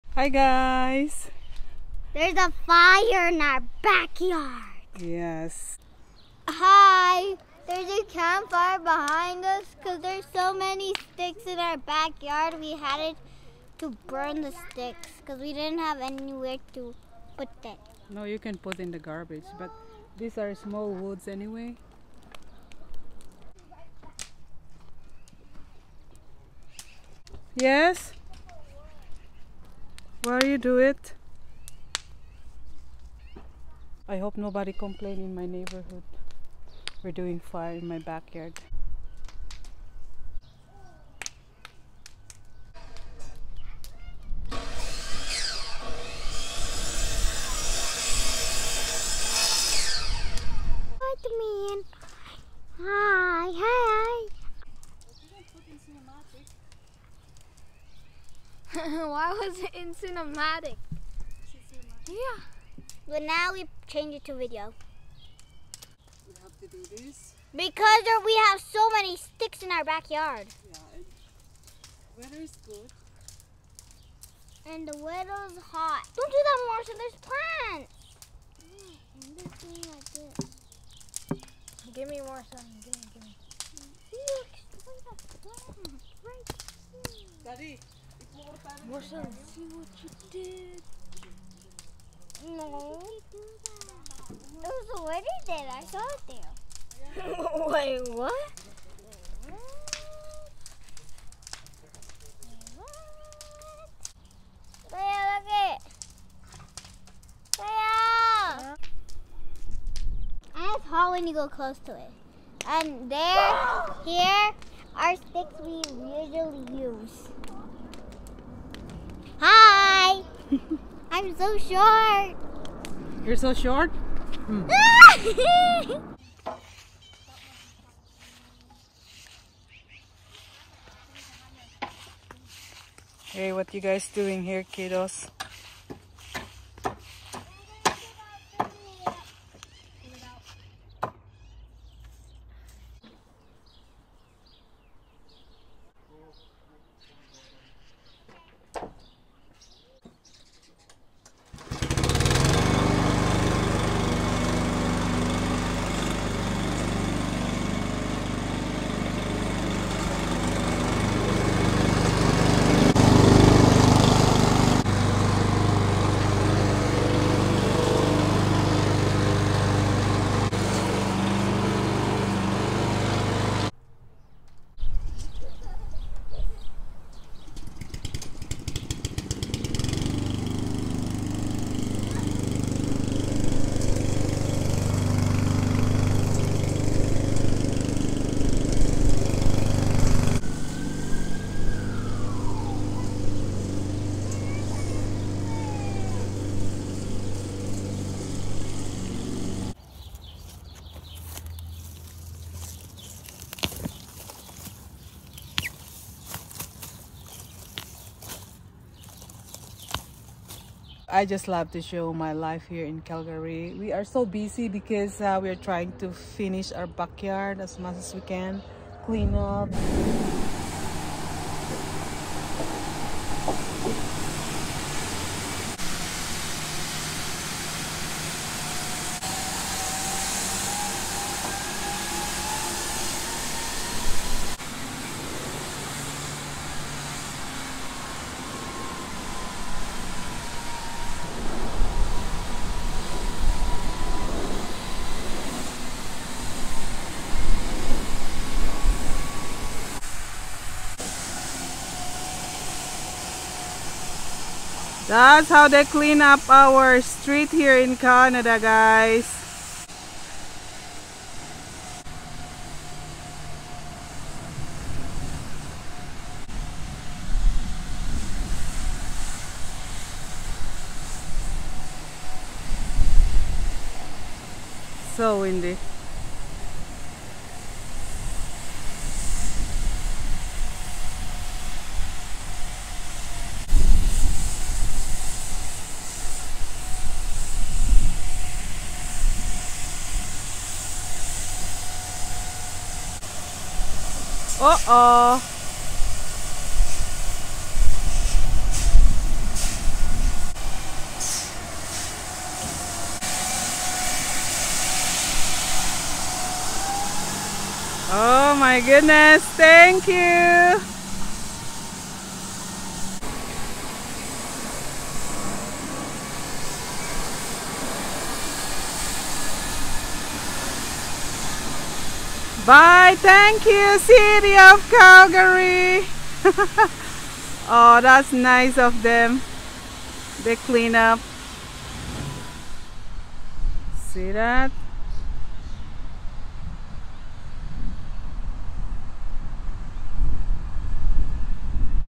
WEEKEND Life IN Canada + city of Calgary street spring cleaning | A day in our life | sarah buyucan
Family weekend in Calgary
-Calgary street cleaning/sweeping
-a day in our life family Vlog
-etc…

I love to share my life reality in Canada, lifestyle, family vlog,adventure etc…to all aspirants immigrants

Mabuhay🇵🇭🙏😊Merci 🇵🇭🇨🇦🙏😊